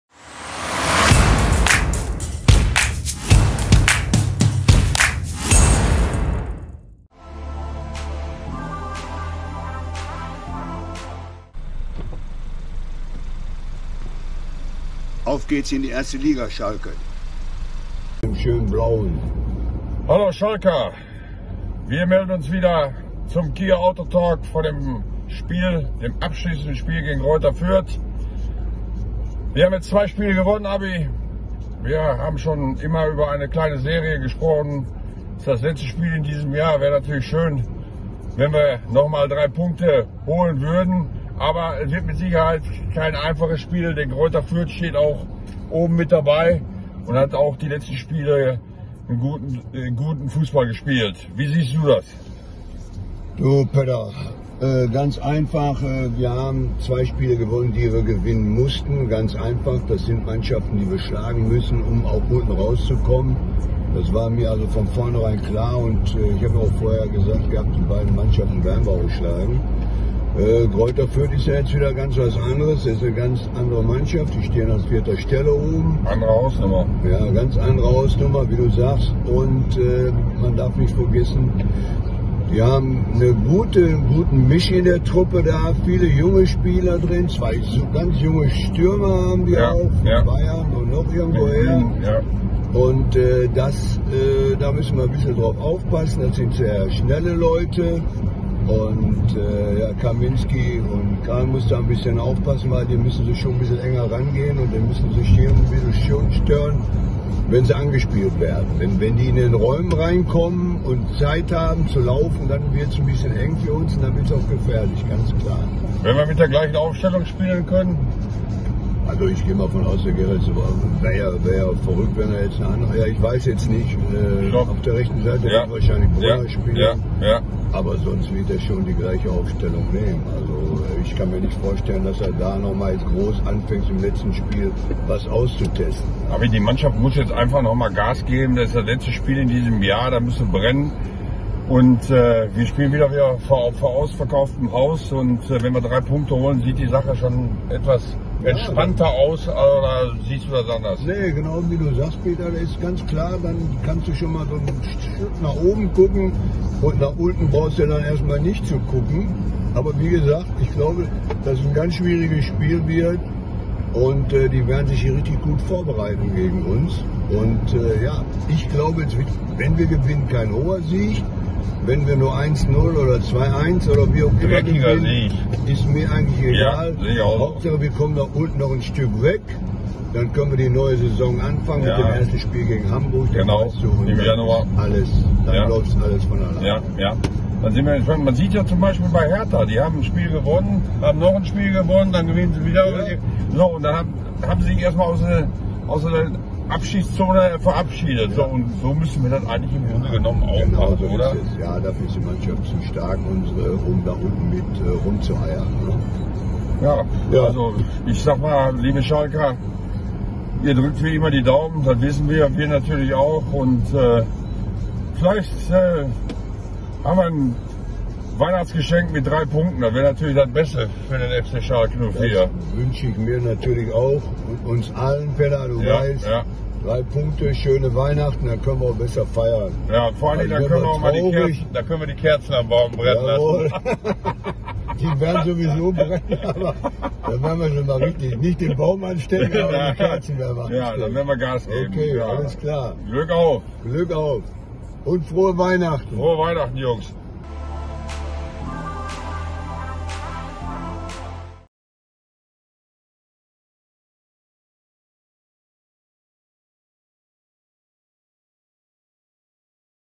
Fussball Legenden Auto Talk - FC Schalke 04 gegen Greuther Fürth
Willkommen beim Fussball Legenden Auto Talk mit Rüdiger Abramczik mit seiner Einschätzung für das Spiel gegen Greuther Fürth. Abi fährt zum Heimspiel und gibt seinen Tipp ab. Am Montag dann wieder wie gewohnt der Legenden Talk mir Rüdiger und Klaus Fischer.
Wir danken für die freundliche Unterstützung vom KIA Autohaus Engbert aus Datteln